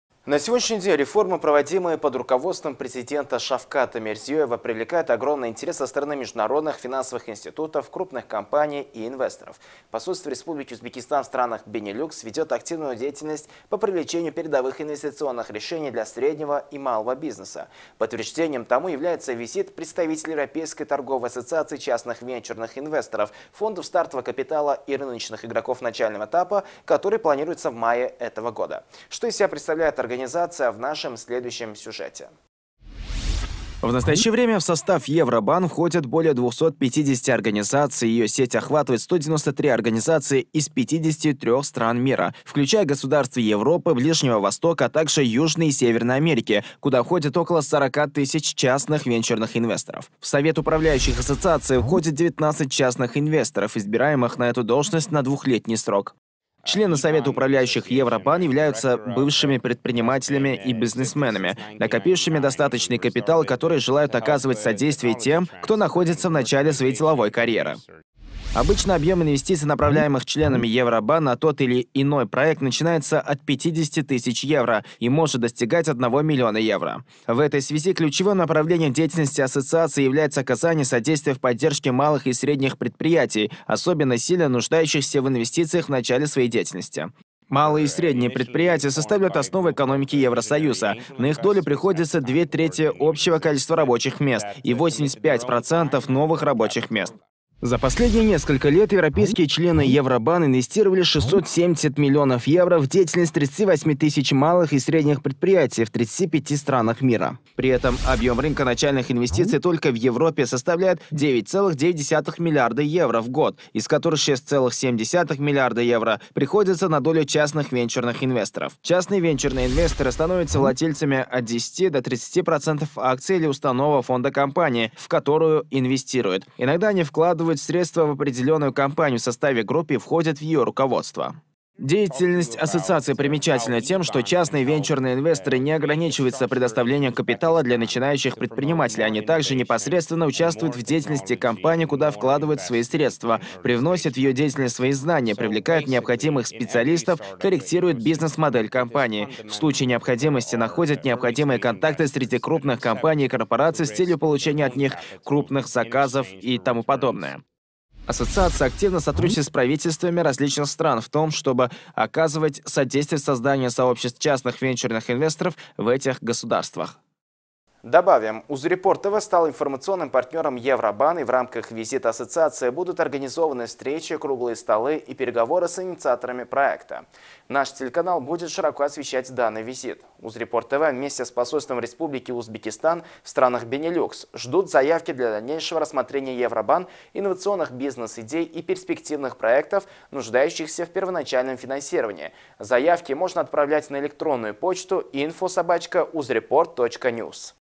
Венчурные инвесторы Европы профинансируют бизнес-идеи в Узбекистане
UZREPORT TV вместе с посольством Республики Узбекистан в странах Бенилюкс ждут заявки для дальнейшего рассмотрения Европейской ассоциацией бизнес-ангелов инновационных бизнес-идей и перспективных проектов нуждающихся в первоначальном финансировании.